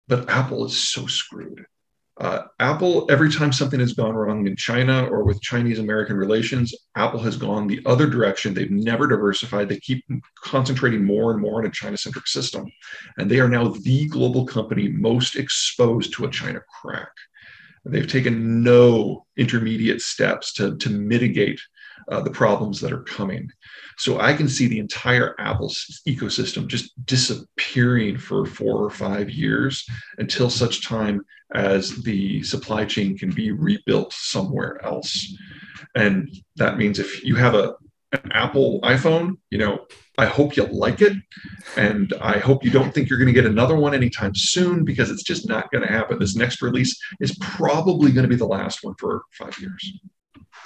"APPLE IS SO SCREWED" - Peter Zeihan
@ZeihanonGeopolitics  discusses his newest book THE END OF THE WORLD IS JUST THE BEGINNING.
Peter Zeihan's newest book
THE ACCIDENTAL SUPERPOWER